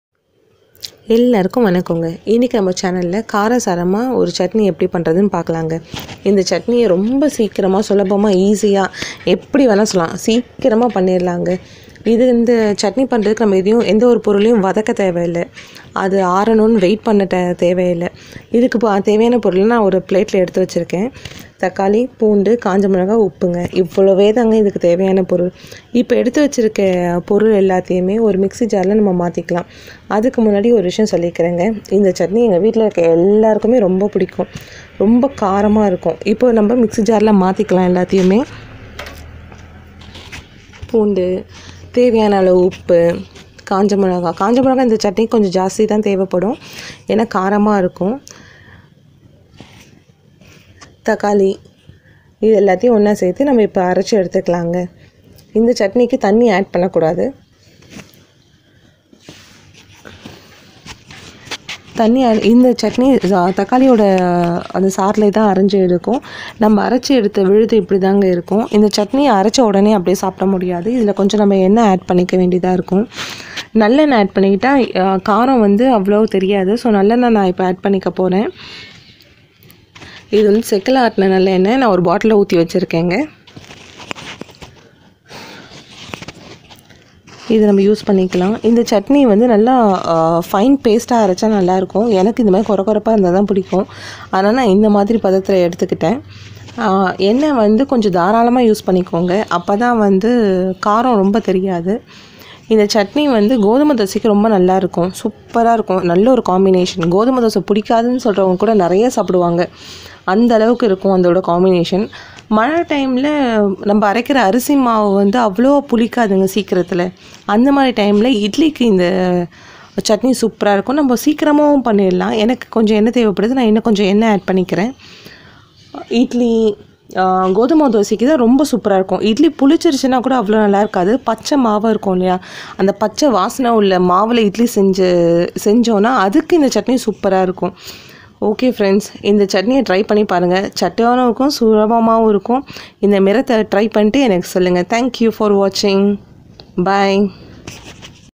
அடுப்பே வேணாம்- மிரட்டல் மிரம்|Cooking Without Fire Chutney in Tamil | 2 minutes Quick Kaara Chutney
The best combination for Wheat Dosa !!